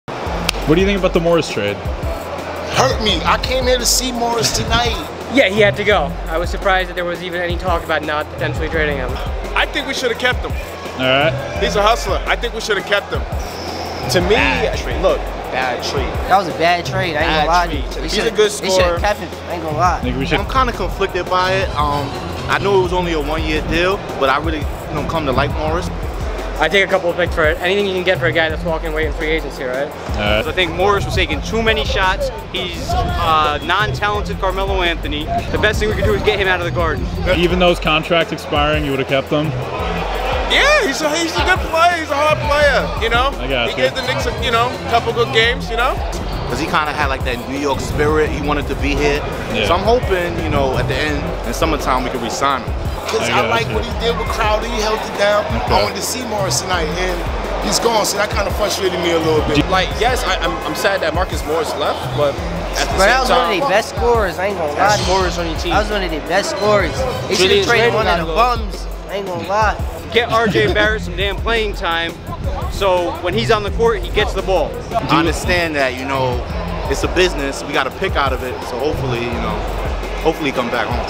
New York Knicks Fans React To The Marcus Morris Knicks Clippers Trade
🔥Get 50% Scotch Porter Premium Beard Collections 🔥

🏀PODCAST LINKS! 🏀

2 ) 🔽Watch These Knicks Video Next!🔽